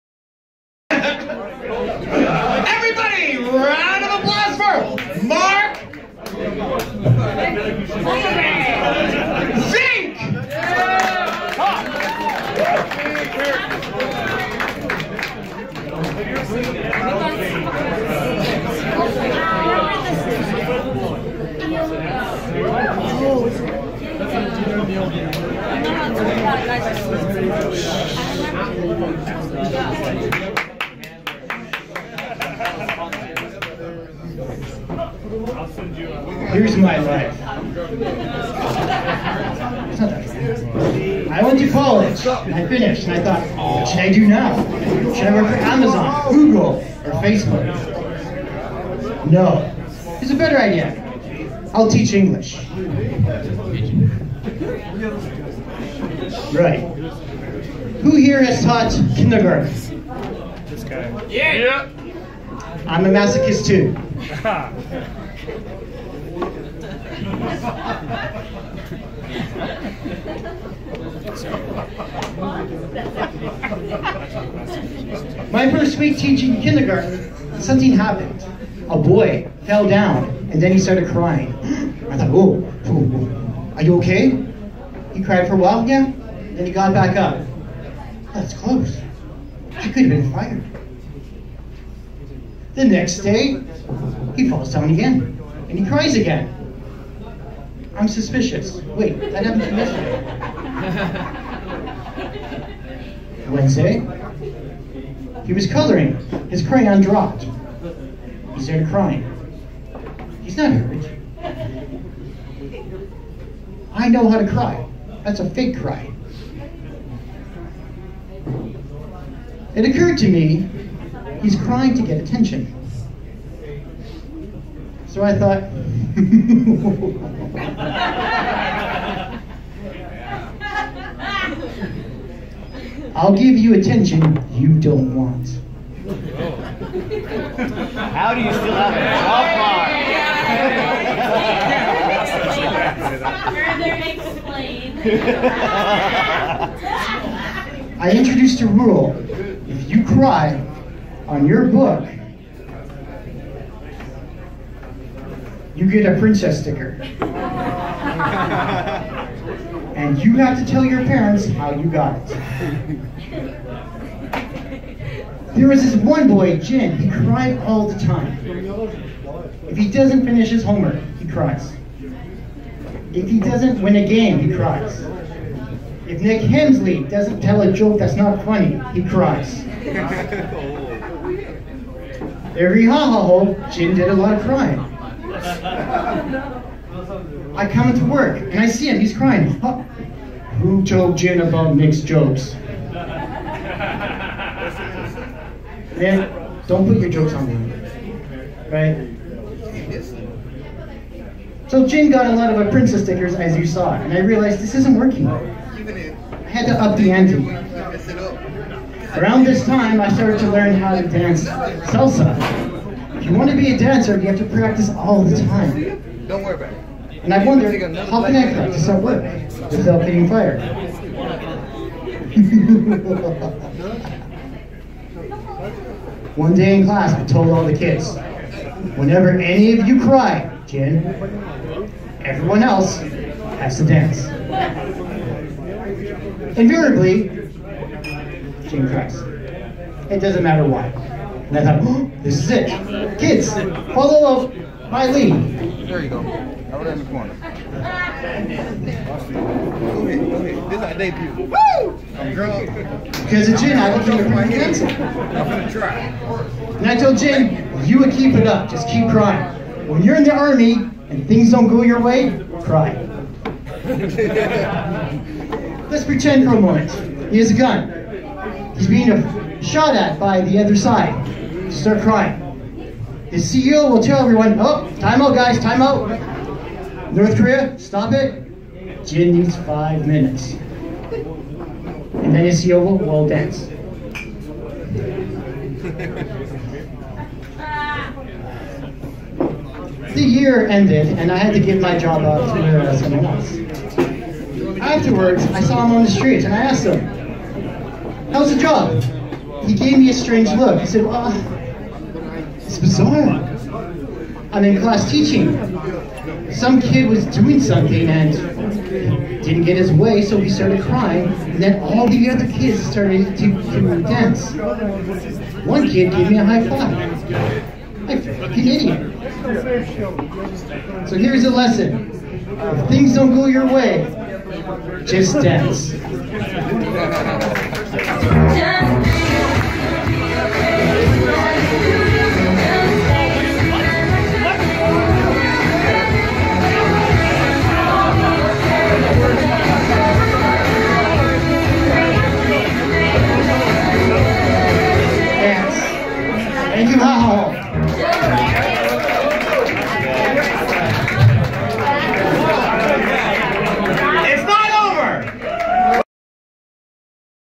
Comedy Routine at Gwangan HQ (9): Just Cry
In the Winter of 2019, I revisited a comedy gimmick I initially used seven years previously (but had not filmed).

It entails my early teaching days, and how some children would incessantly cry.  As a countermeasure, I implemented a technique to give them attention they don't want.